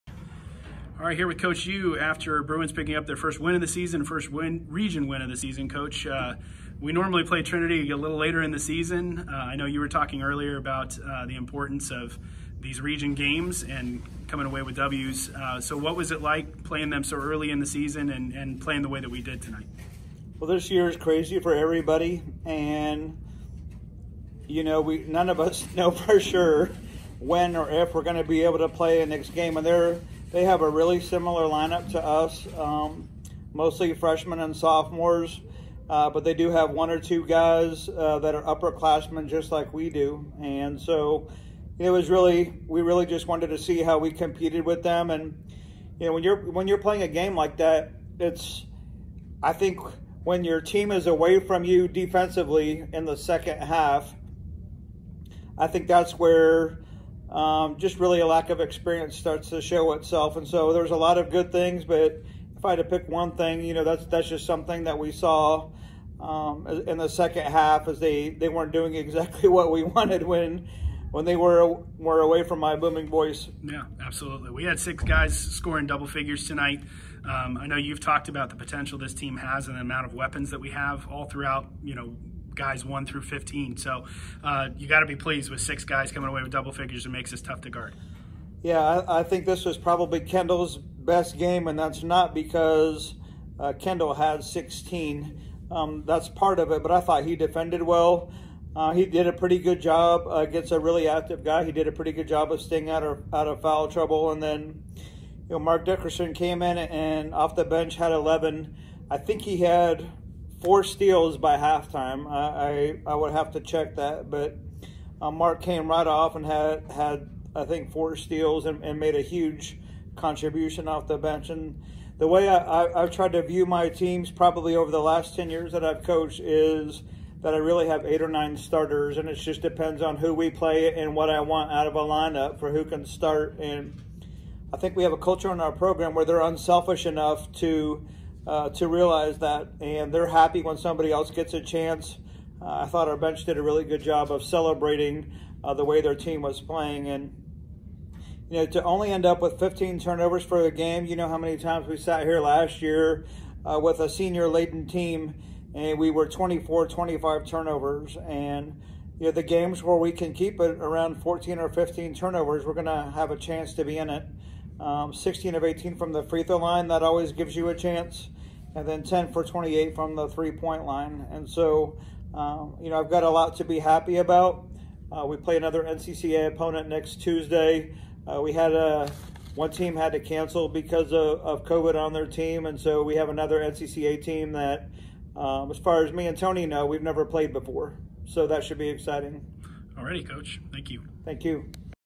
Postgame Interview - Men’s Basketball - 11/14/20
Coach U talks about the Bruins win over Trinity Baptist College.